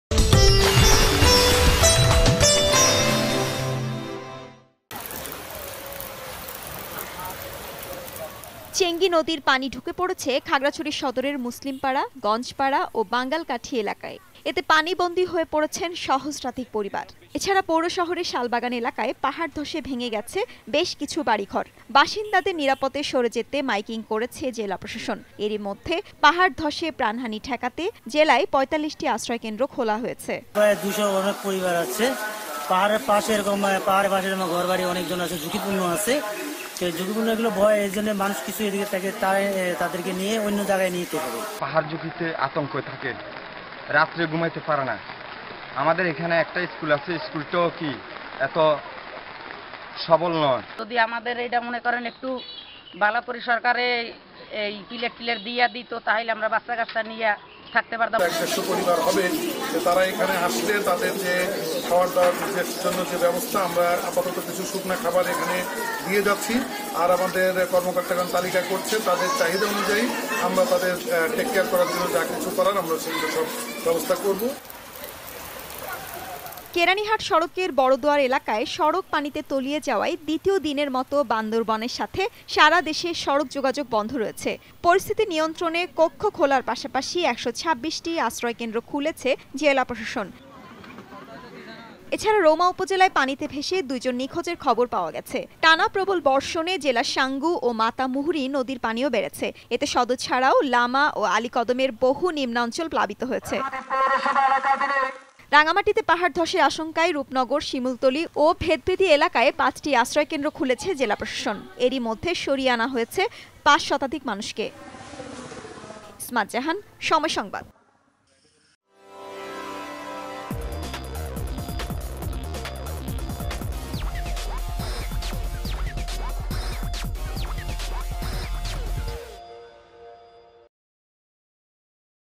সুনামগঞ্জ ও কুড়িগ্রামে নদ-নদীর পানি বেড়েছে
গত কয়েকদিনের ভারী বৃষ্টি ও উজান থেকে নেমে আসা ঢলে সুনামগঞ্জ ও কুড়িগ্রামে নদ-নদীর পানি বেড়েছে।